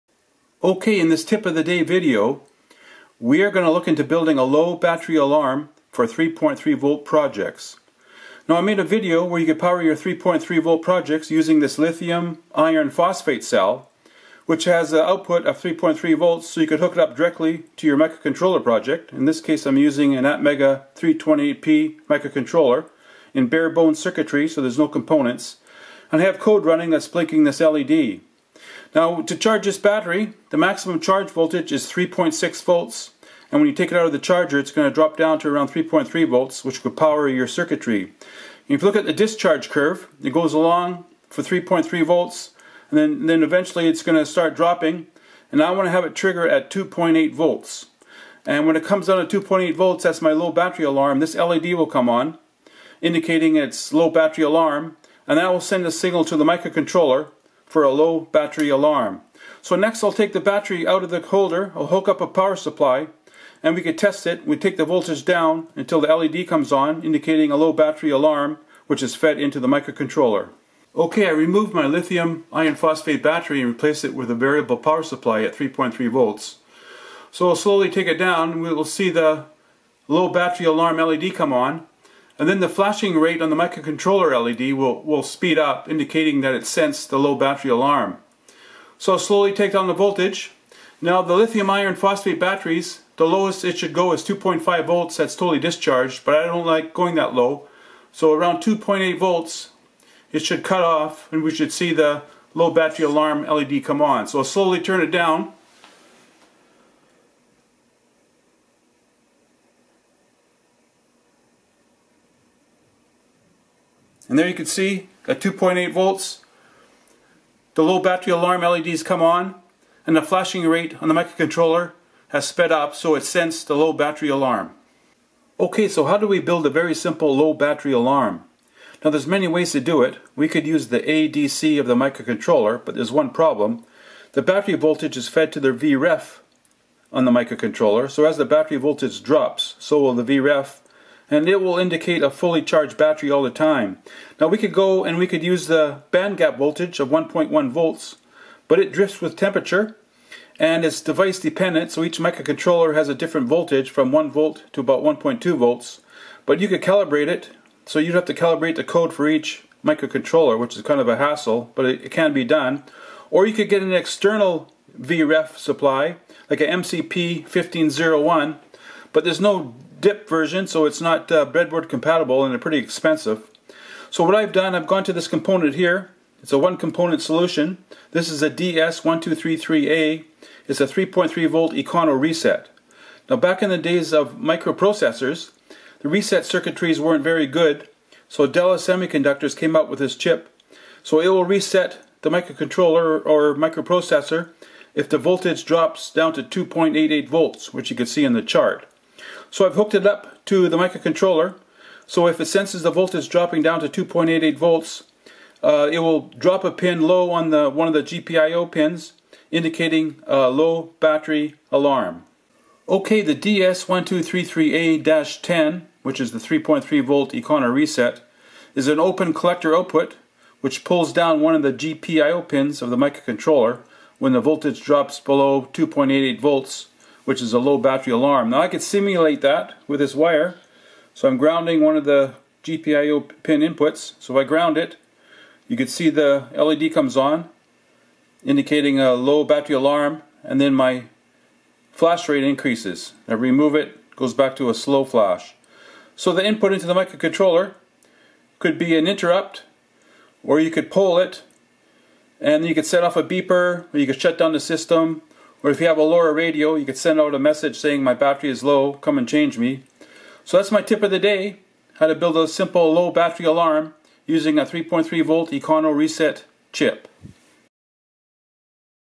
Tip Of The Day / Low Battery Alarm / DS1233A-10
This video will show how to build a single component low battery alarm for 3.3v projects.